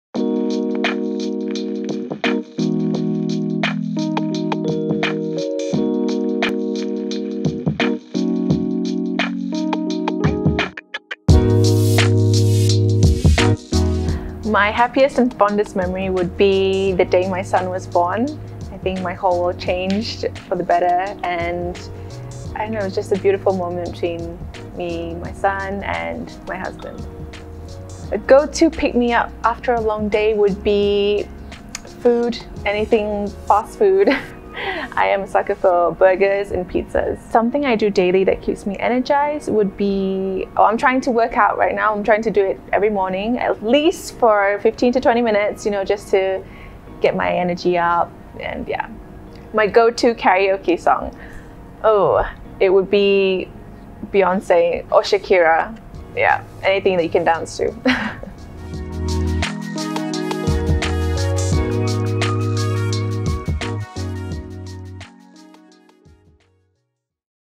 Behind the Scenes With Zahirah Macwilson
Actress and model Zahirah Macwilson is our March 2022 cover star, wearing gorgeous watches and jewellery from @chopard.